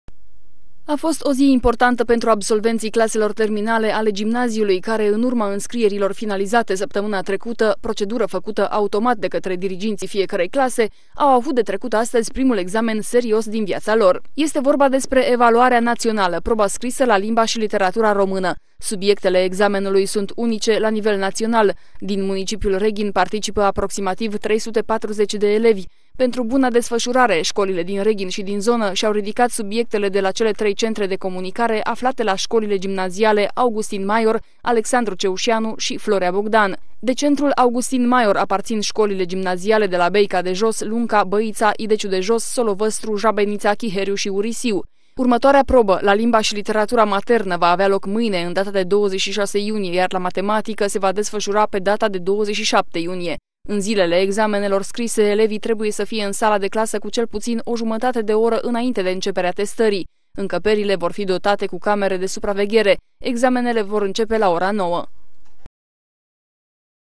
PRIMA PROBĂ A EVALUĂRII NAŢIONALE
A fost o zi plină de emoții pentru absolvenții claselor a VIII-a care au avut de susținut astăzi proba la limba și literatura română a evaluării naționale. Examenul a început la ora 9:00 iar sălile de clasă au fost dotate cu camere de supraveghere.